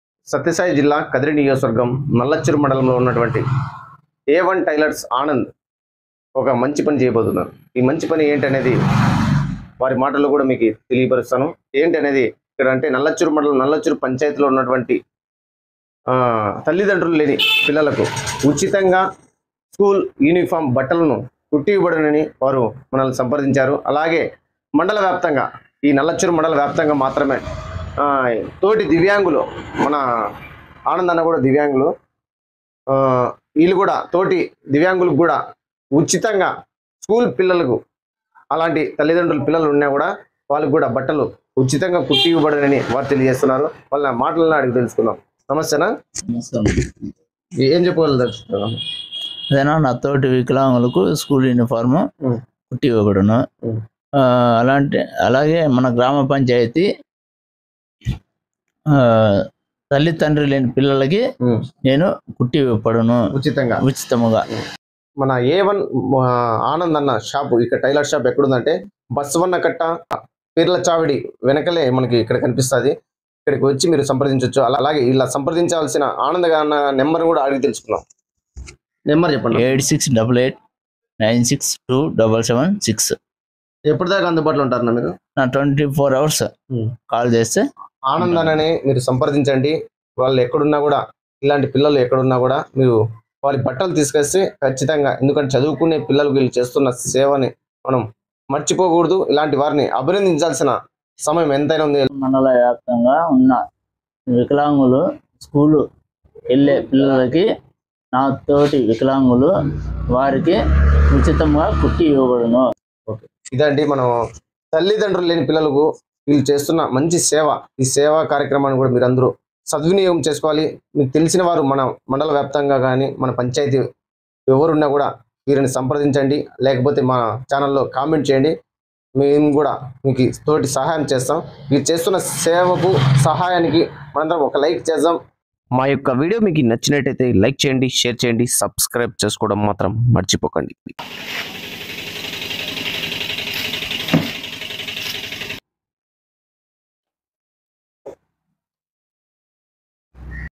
నల్లచెరువు మండలంలోని తల్లిదండ్రులు లేని పిల్లలకు ఉచితంగా స్కూల్ బట్టలు కుట్టి ఇవ్వబడును
దివ్యాంగుల తల్లిదండ్రుల పిల్లలకు  మరియు తల్లిదండ్రులు లేని పిల్లలకు ఉచితంగా స్కూల్ యూనిఫామ్  ఆనంద్ ఏ వన్  టైలర్ షాప్ లో  కుట్టి ఇవ్వబడును